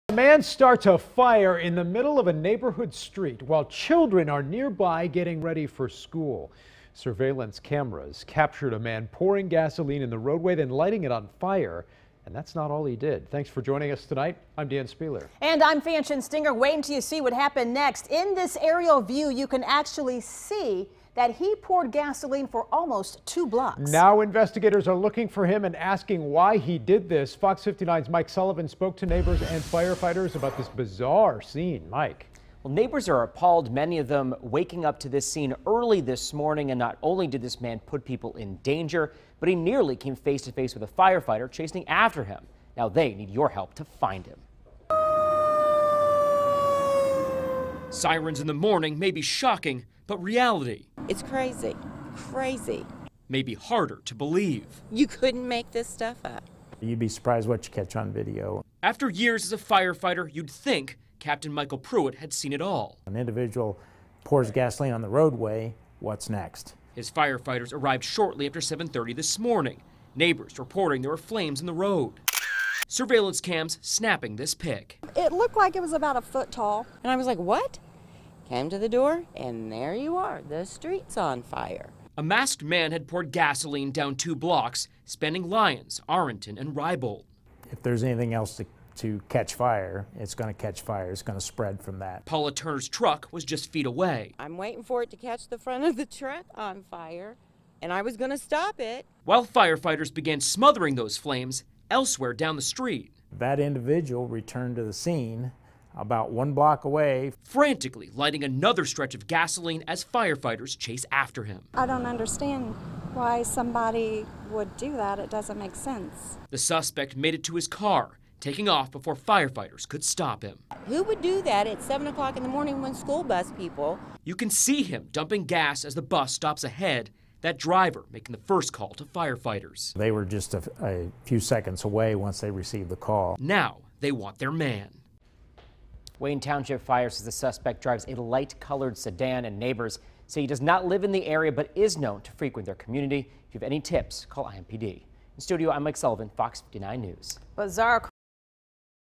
Caught on camera: street arsonist in Wayne Township
A man on the west side of Indianapolis was captured on camera setting fire to the street.